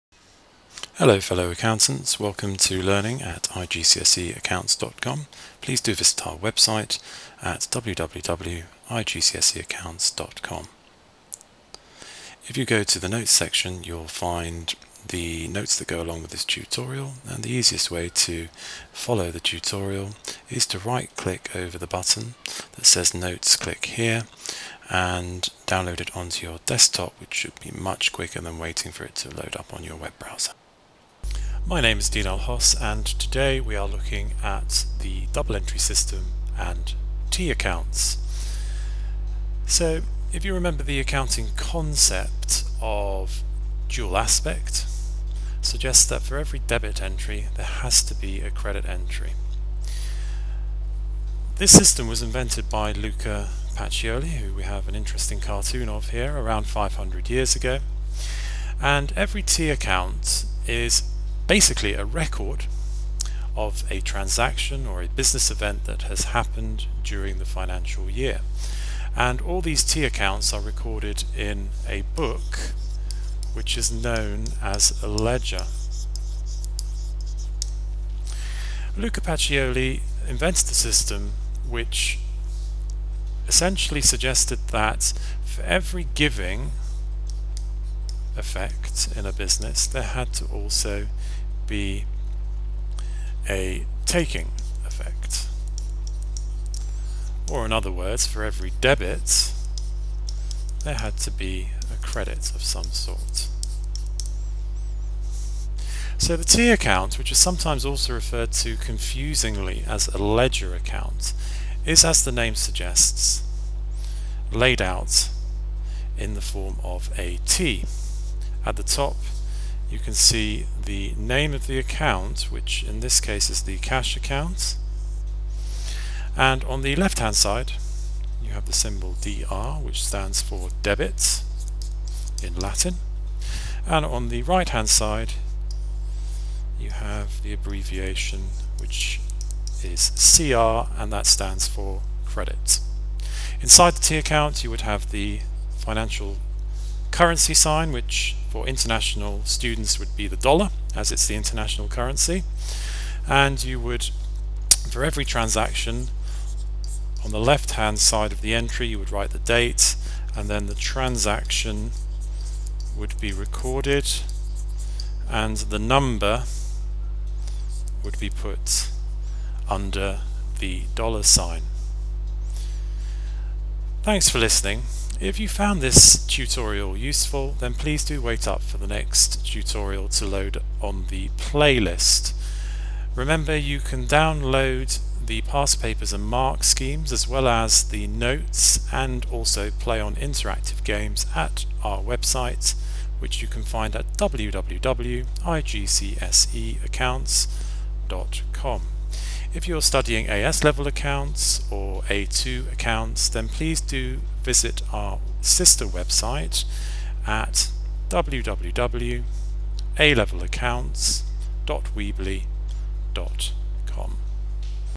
IGCSE & GCSE Accounts - Understanding IGCSE Double Entry and the Ledger 'T' Account
Learn how to layout an IGCSE Ledger or 'T' Account. After watching this tutorial you will be able to understand what the abbreviation Dr and Cr stand for as well as the meaning of a Ledger Book. Please check out our youtube channel for other tutorials and our